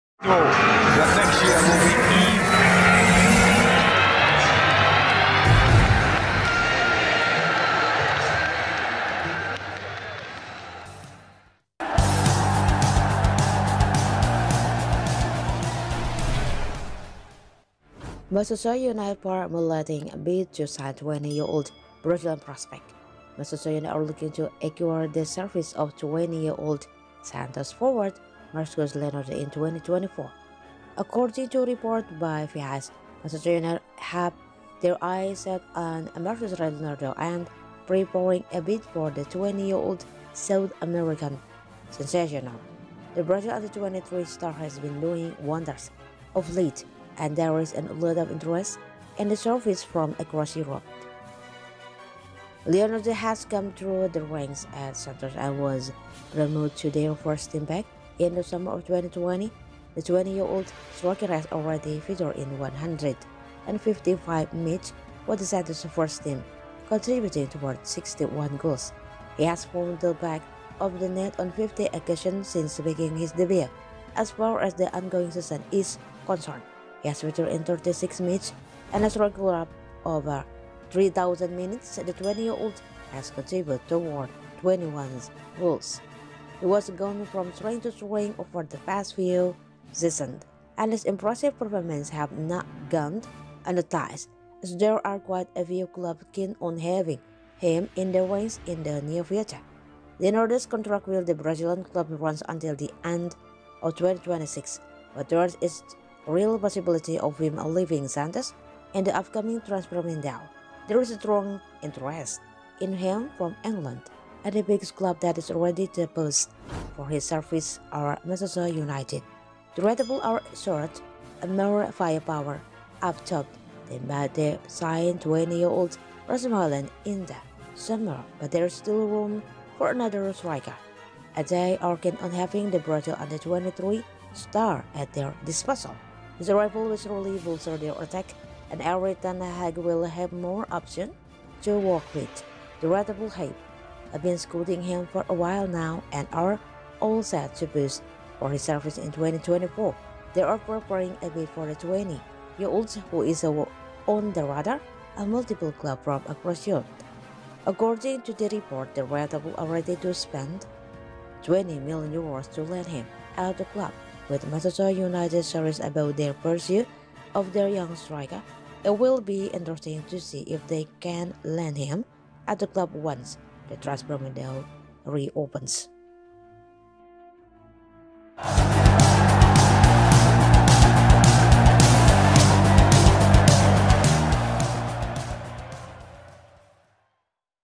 BID SUBMITED: Manchester United formulating bid to sign 20-year-old Brazilian prospect
Manchester United formulating bid to sign 20-year-old Brazilian prospect